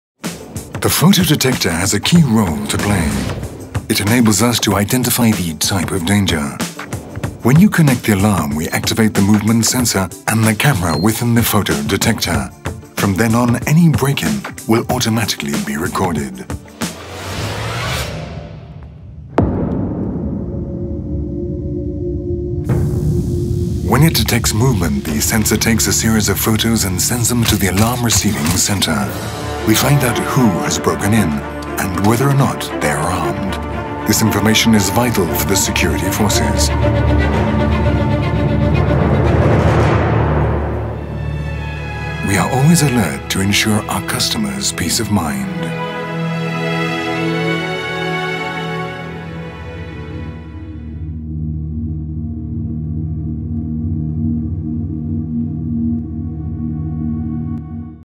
The Photo Detector
The PIR Sensor is a powerful Photo Detector with an integrated flash, so it can take photographs even in total darkness. When our motion sensor device detects an intruder, it automatically sends an image sequence to our Alarm Receiving Centre. Our team then analyses the captured images by the Photo Sensor and contacts the Guard Response if needed.

The images are recorded and kept in Verisure's Data Processing Centre. They will only be released upon request from either the customer or the Guard Response.